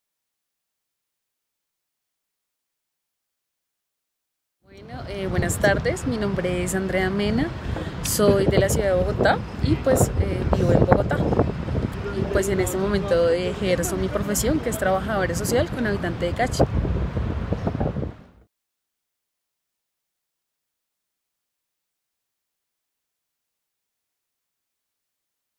EPT: Presentación Andrea Mena
Andrea Mena se presenta a la comunidad de EPT. Si quieres conocer la anécdota de pelos de Andrea sigue este enlace